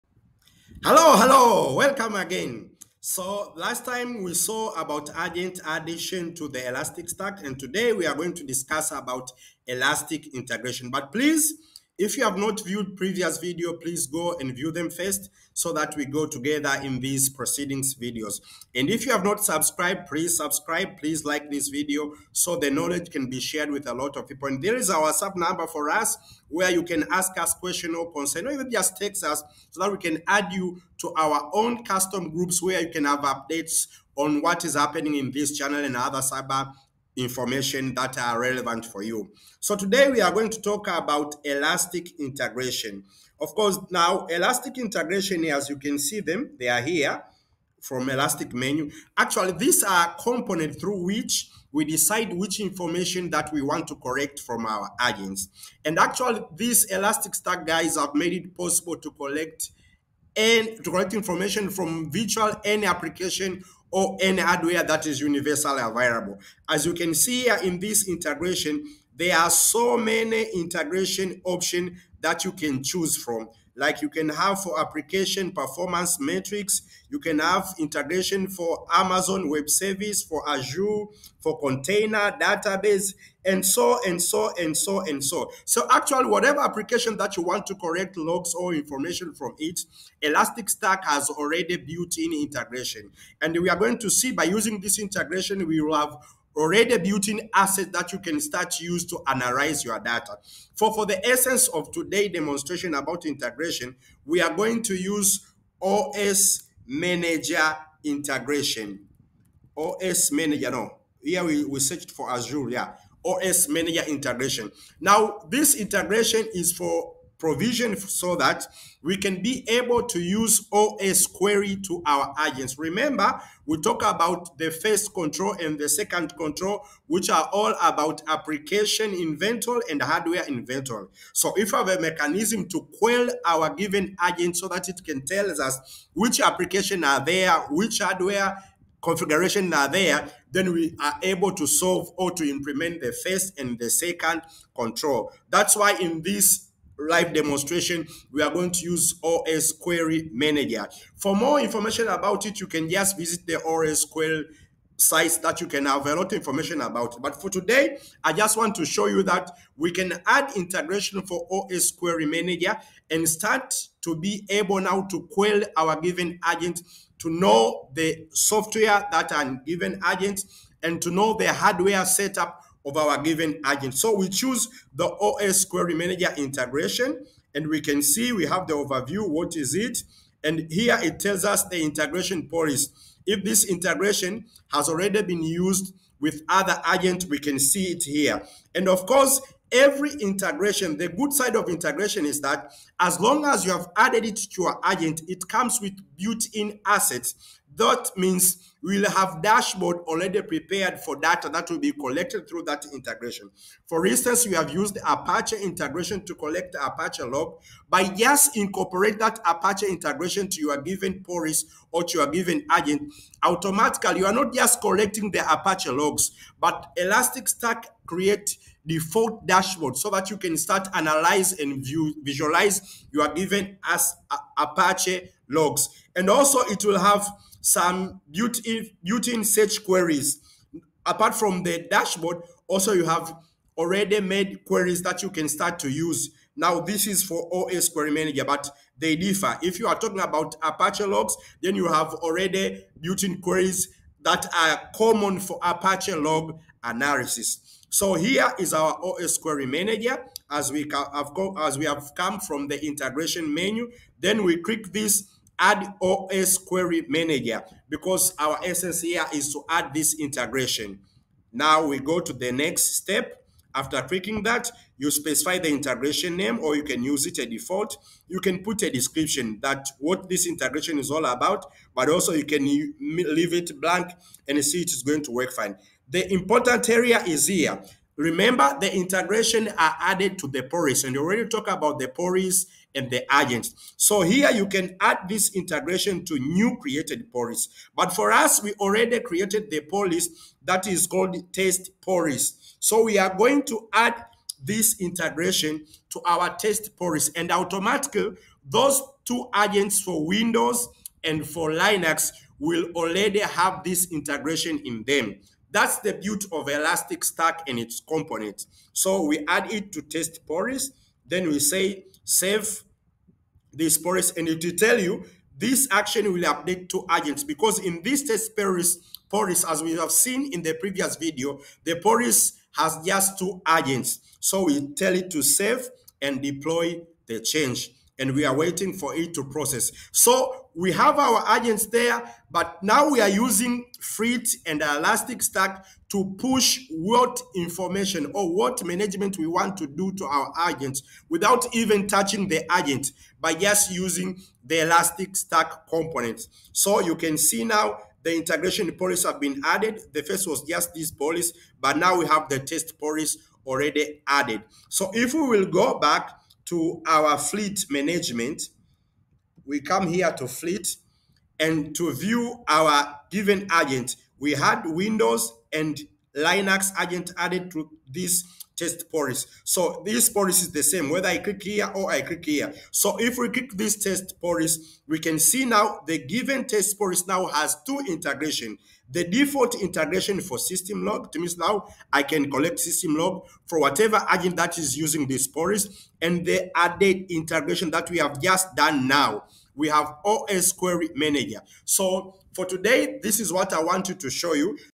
Elastic Stack - Integrations
In this video im talking about what elasti integrations are and their role in elastic stack eco system. if you have any concern or query about cyber security in general or content of this video whatsapp msg us at